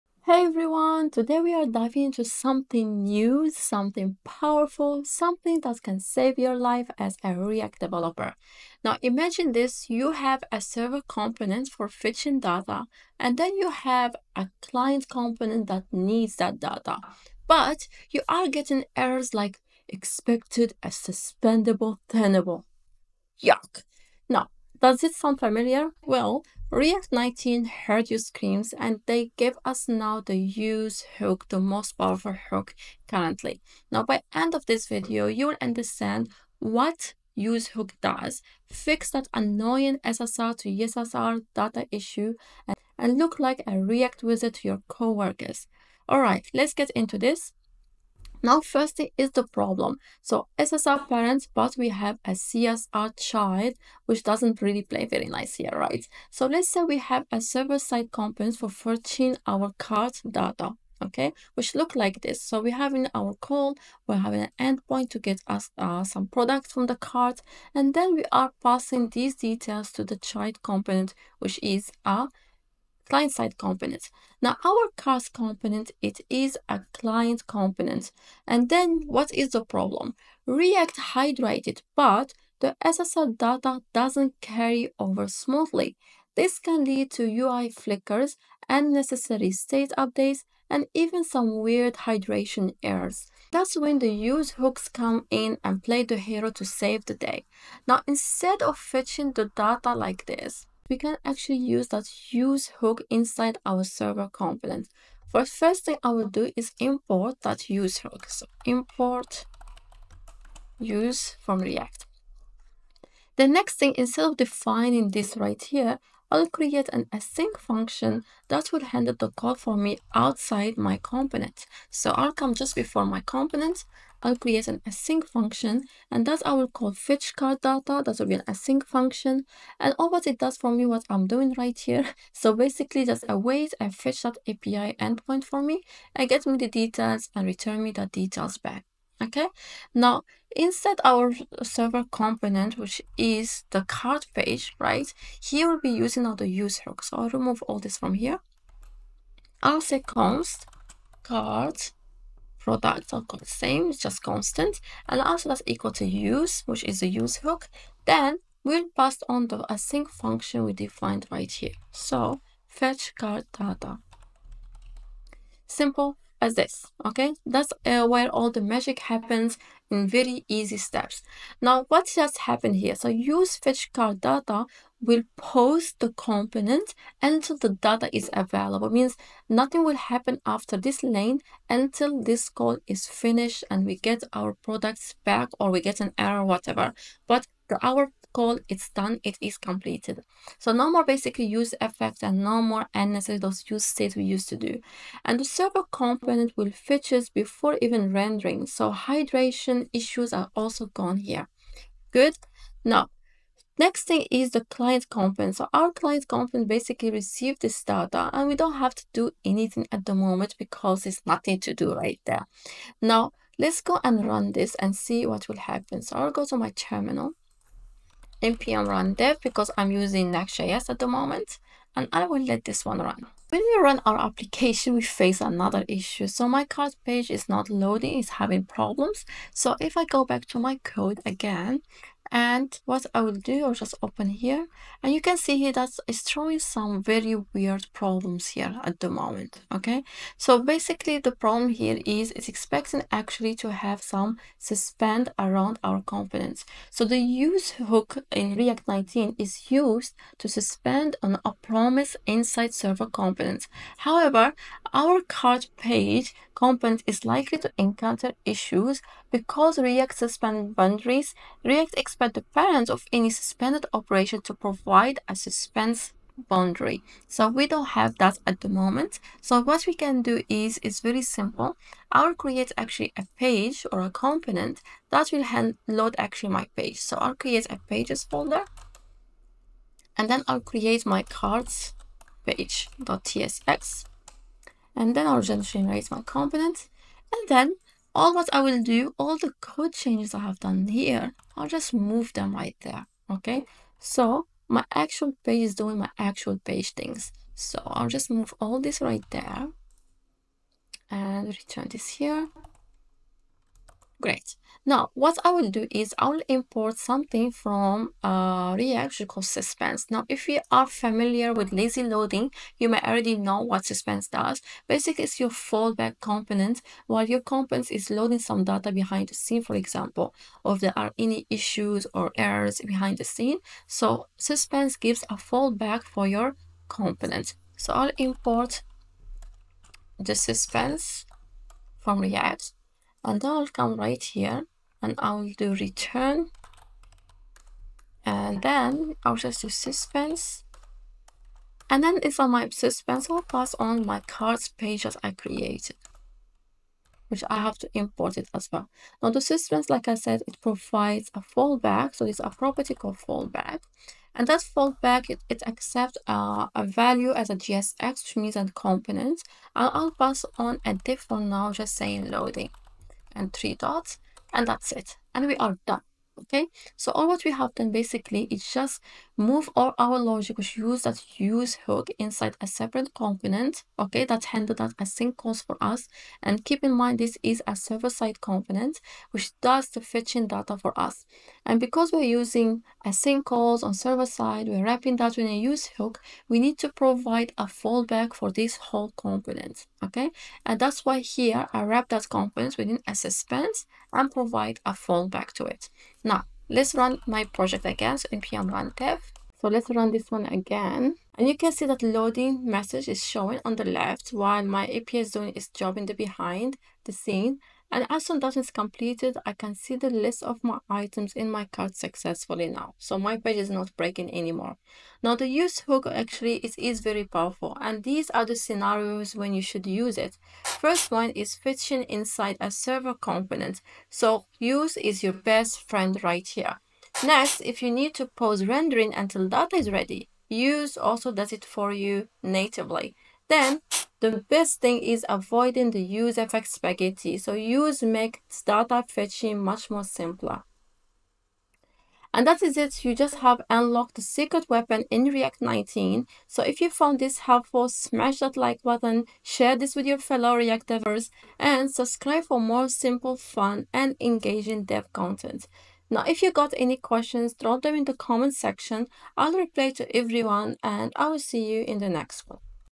React 19’s Game-Changer: The use Hook Explained
🚀 React 19 introduced a game-changer: the use hook! If you've ever struggled with data fetching in a Server Component (SSR) while passing data to a Client Component (CSR)—this video is for you!

🔥 In this quick but powerful tutorial, you'll learn:
✅ What the use hook is and why it’s a BIG deal.
✅ How it eliminates useEffect spaghetti and prevents hydration issues.
✅ The best way to fetch data in a Server Component and pass it smoothly to a Client Component.
✅ When to use use and why it saves time and performance in Next.js.

No more flickering UI. No more hydration errors. Just clean, efficient, and powerful React 19 code! 🚀

💡 Found this useful? Your support helps us bring more engaging, beginner-friendly React & Next.js content!